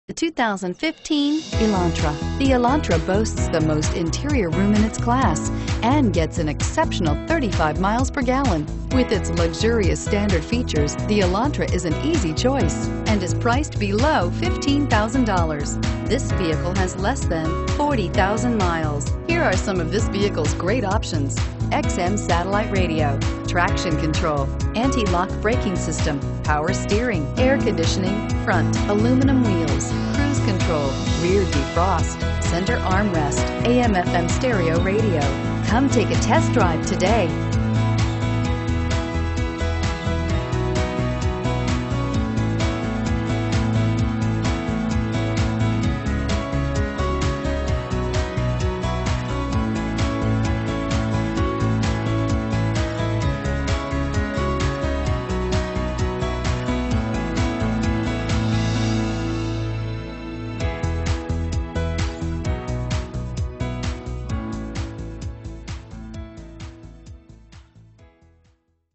2015 Hyundai Elantra Cookeville TN M1339
2015 Hyundai Elantra SE

Carlen Chevrolet
330 West Spring Street

New Arrival! CarFax One Owner! Satellite Radio, Aux Audio Input, This Hyundai Elantra gets great fuel economy with over 38.0 MPG on the highway! ABS Brakes Satellite Radio Front Wheel Drive Please let us help you with finding the ideal New, Used, or Certified vehicle.